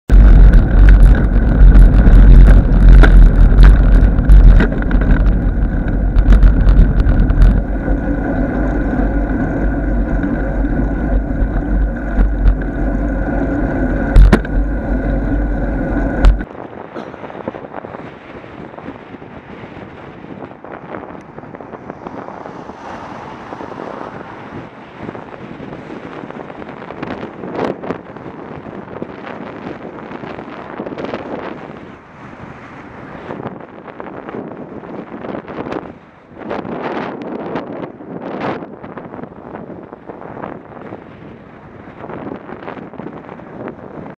SP10URD aborted overtake
Every time I come down this road, regardless of whether I'm driving or on the bike, I take the opportunity to look ahead on the road just in case there's something after the corner that might come as an unpleasant surprise. The driver of SP10URD certainly didn't look and neither did the driver of the white van ( I don't think any of them looked ).

Friday 9th January 2015 approx 08:30